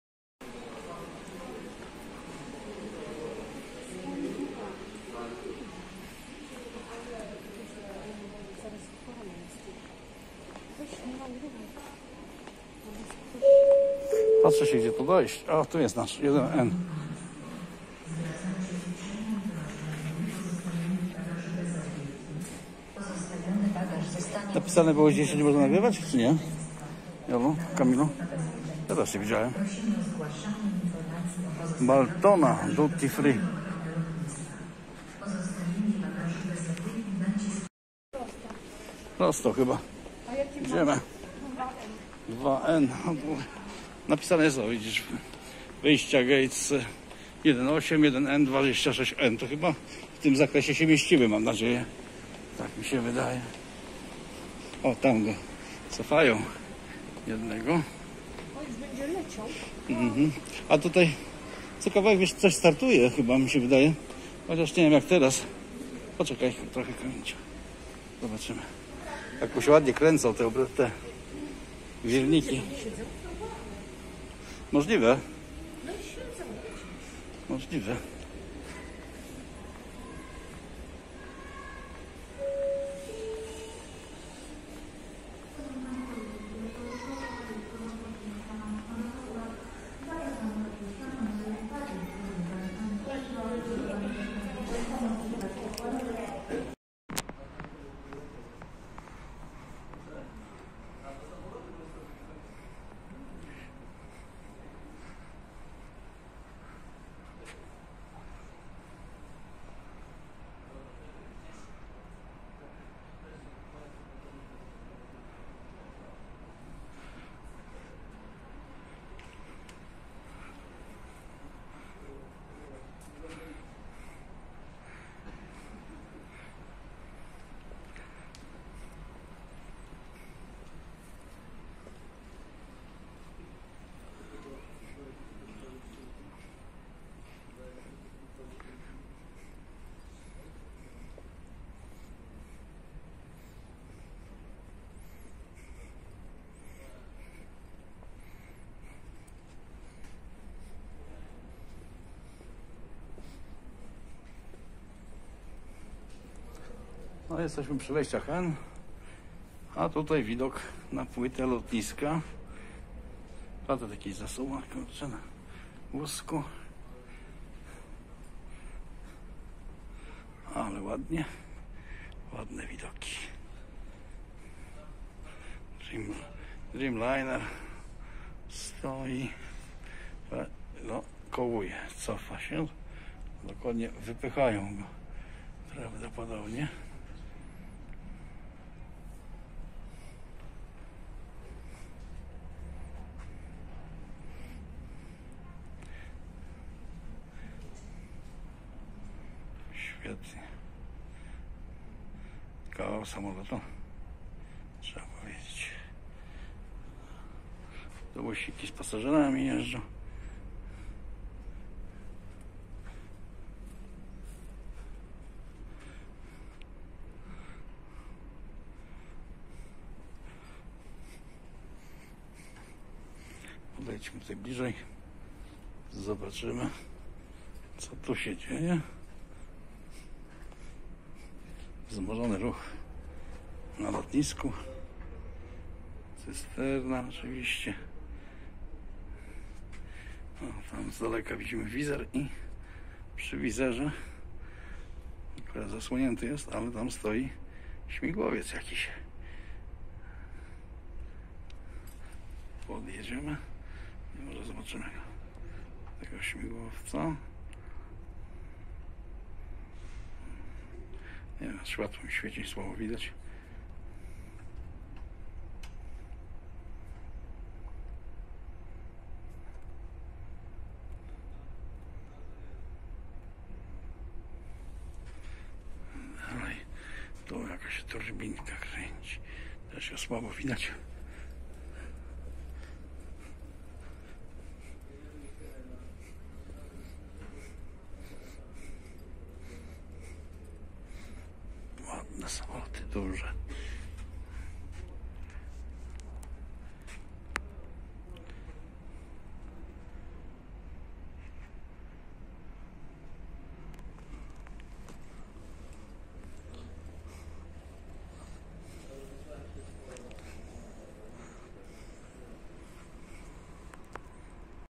Lotnisko Chopina bramka 2N lot RR5363 Antalya linie Buzz - Turcja 07.2021 rok
Oczekujemy na lot Warszawa-Antalya Gate nr 2N. Lnie Buzz (Ryanair). Lecimy do Turcji z TUI. Hotel Elysee Alanya.